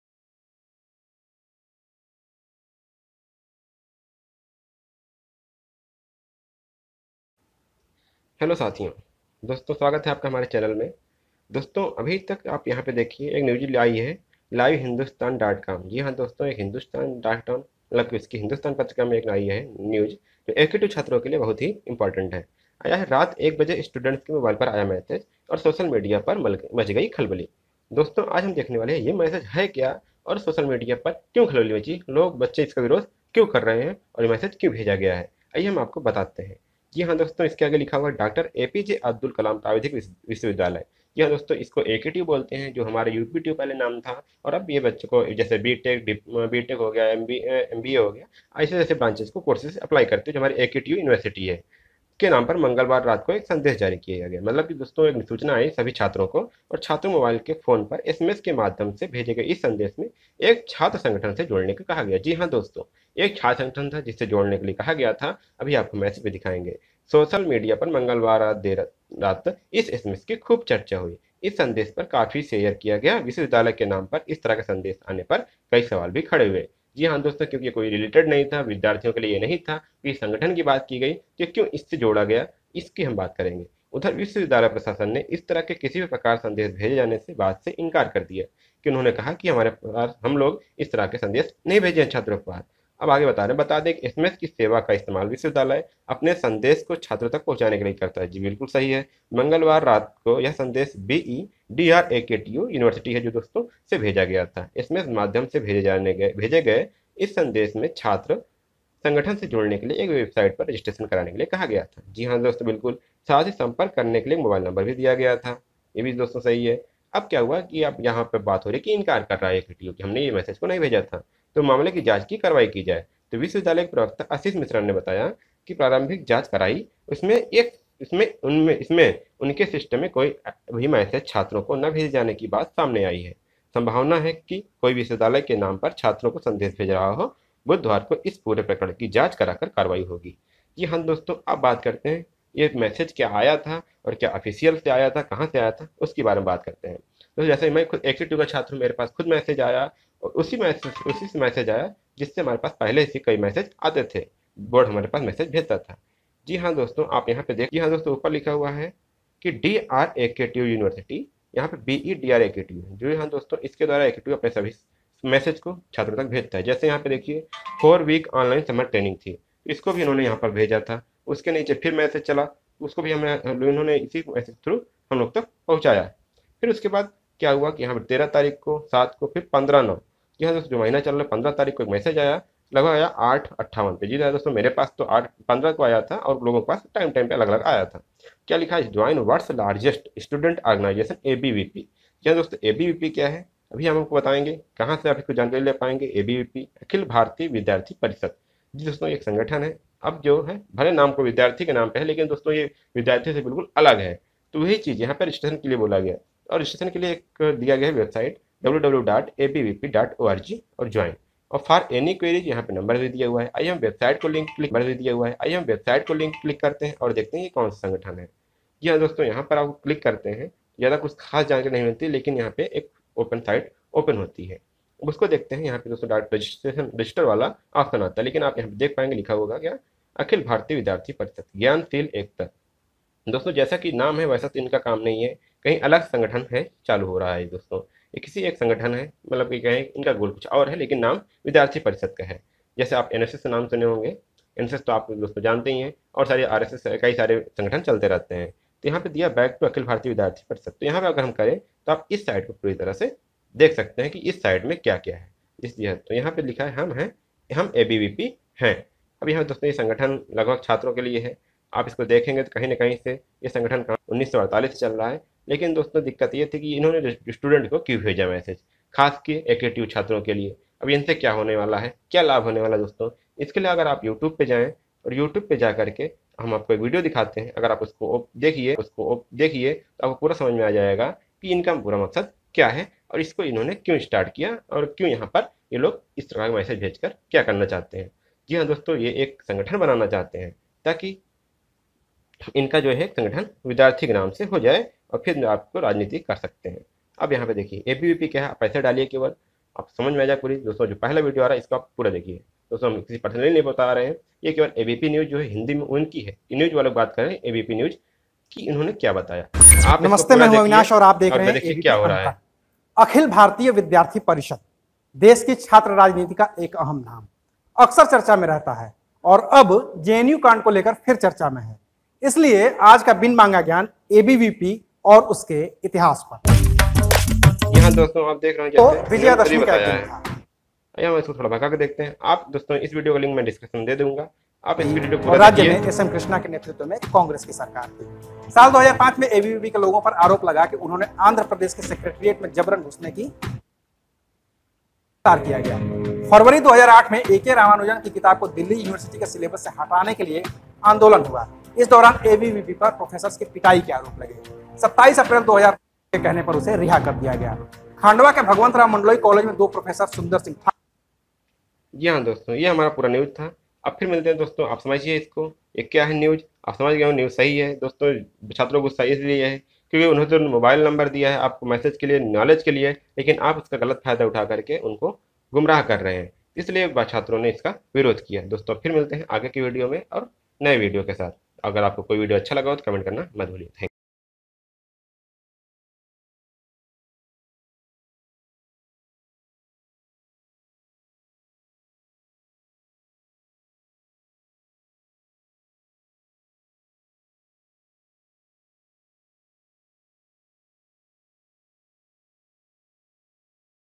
AKTU big news 🎇 रात 1 बजे स्टूडेंट्स के मोबाइल पर आया मैसेज, सोशल मीडिया पर मच गई खलबली
रात 1 बजे स्टूडेंट्स के मोबाइल पर आया मैसेज, सोशल मीडिया पर मच गई खलबली

डॉ. एपीजे अब्दुल कलाम प्राविधिक विश्वविद्यालय (एकेटीयू) के नाम पर मंगलवार रात एक संदेश जारी किया गया। छात्रों के मोबाइल फोन पर एसएमएस के माध्यम से भेजे गए इस संदेश में उन्हें  एक छात्र संगठन से जुड़ने को कहा गया है। सोशल मीडिया पर मंगलवार देर रात इस एसएमएस की खूब चर्चा रही। इस संदेश को काफी शेयर किया गया। विश्वविद्यालय के नाम पर इस तरह के संदेश आने पर कई सवाल भी खड़े हुए।
 उधर, विश्वविद्यालय प्रशासन ने इस तरह के किसी भी प्रकार के संदेश भेजे जाने की बात से इनकार कर दिया है। बता दें कि एसएमएस की सेवा का इस्तेमाल विश्वविद्यालय अपने संदेश को छात्रों तक पहुंचाने के लिए करता है। मंगलवार रात को यह संदेश बीई - डीआरएकेटीयू से भेजा गया है। एसएमएस के माध्यम से भेजे गए इस संदेश में एक छात्र संगठन से जुड़ने के लिए एक वेबसाइट पर रजिस्ट्रेशन कराने के लिए कहा गया। साथ ही, संपर्क करने के लिए एक मोबाइल नंबर भी दिया गया है।
विश्वविद्यालय के प्रवक्ता आशीष मिश्रा ने बताया कि उन्होंने प्रारंभिक जांच कराई। इसमें, उनके सिस्टम से ऐसा कोई भी मैसेज छात्रों को न भेजे जाने की बात सामने आई है। संभावनाएं हैं कि कोई विश्वविद्यालय के नाम पर छात्रों को संदेश भेज रहा हो। बुधवार को इस पूरे प्रकरण की जांच करा कर कार्रवाई होगी।